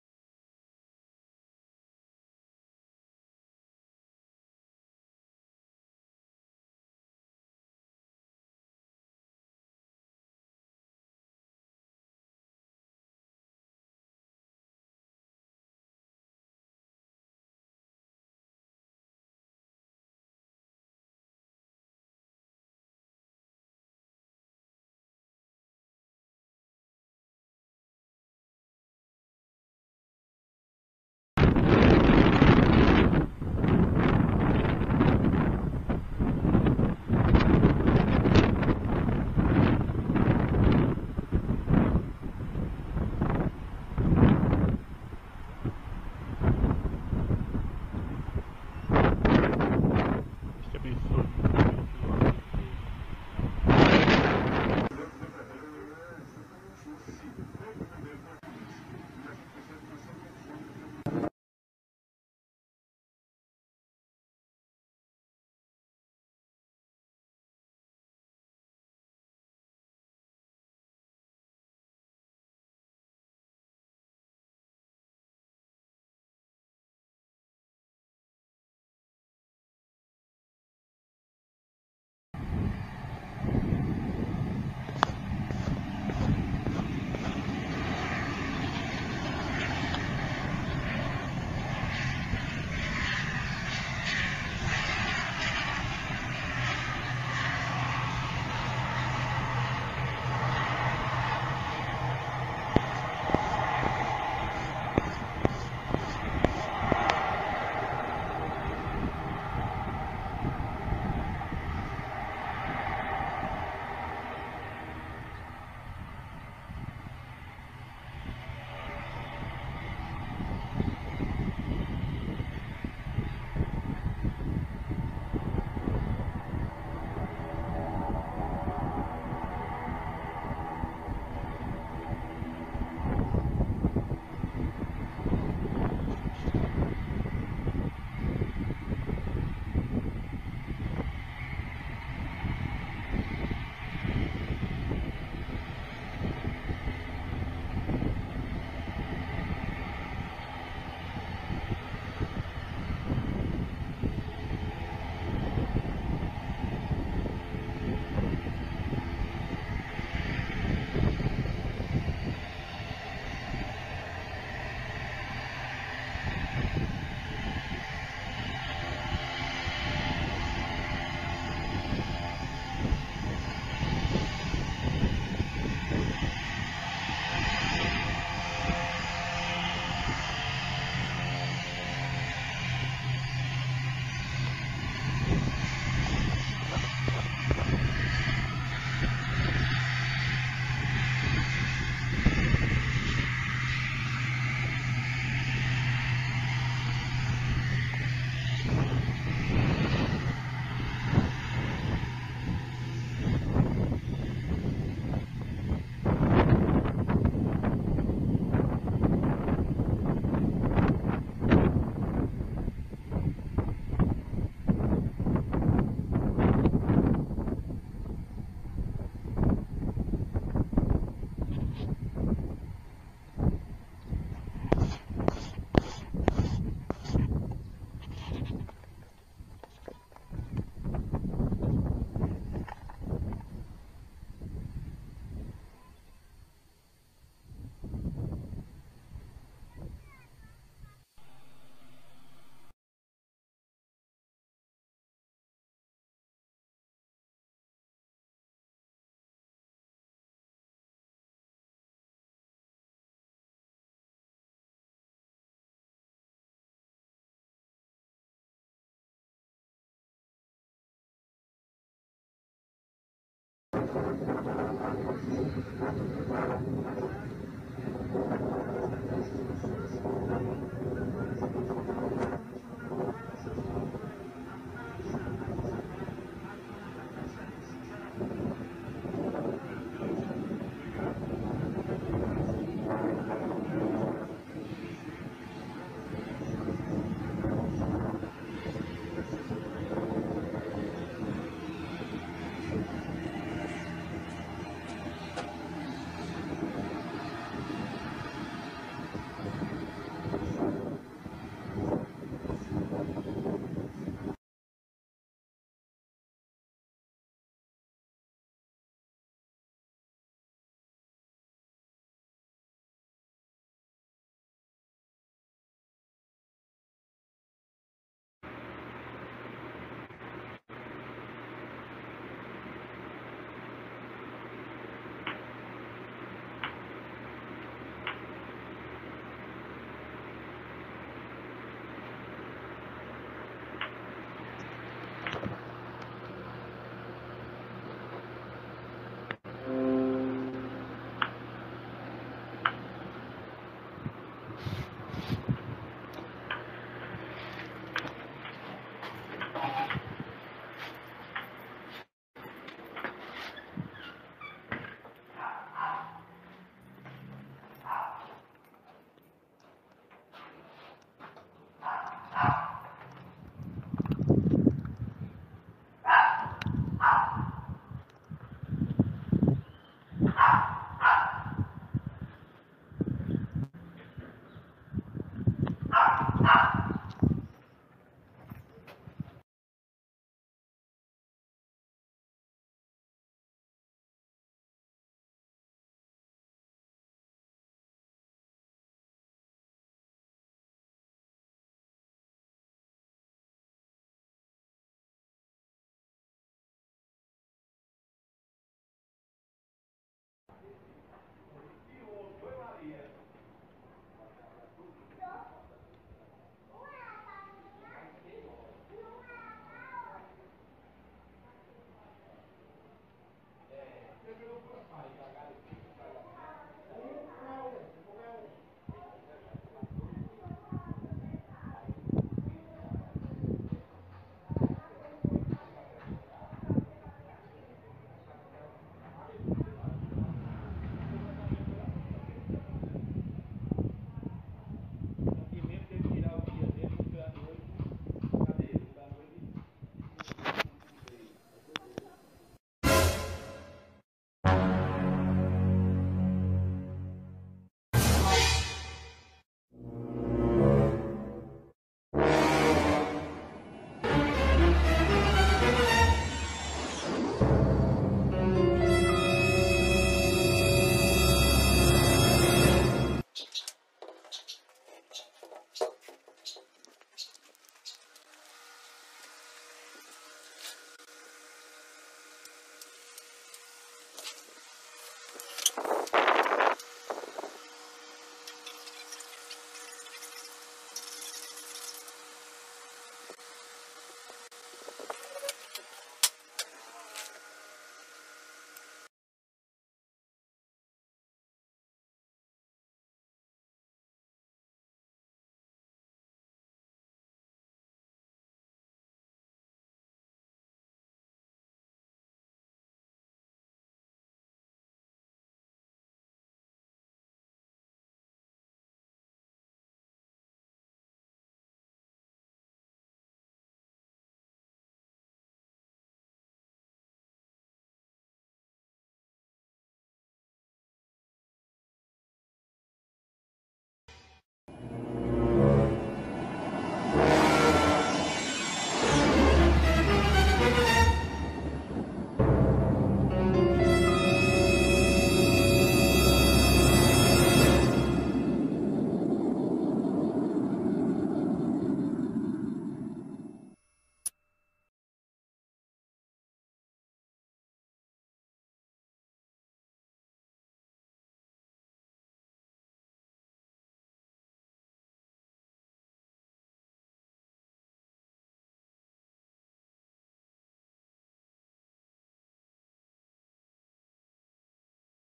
Voo 4108 da Azul Linhas Aéreas filmado por 2 amigos meus em 23/03/2023
AIRCRAFT
ATR 72-600
REGISTRATION
PR-AQW
Vindo de Recife-Pe
E pousando no aeroporto de Paulo Afonso-Ba as 13:33hs,
Livrando a pista de pouso as 13:37hs
Filmagem do pouso na cabeceira14 do aeroporto entre as 13:33hs feita por Washington Luís,
Filmagem do taxi em solo [BACK TRAFFIC ] na cabeceira 32 do referido aeroporto as 13:33hs feita por André Moto taxi,
Filmagem livrando a pista de pouso as  13:51hs feita por Washington Luís,
E eu mesmo finalizando este vídeo em mina casa as 13:35hs filmando o carro dos Correios .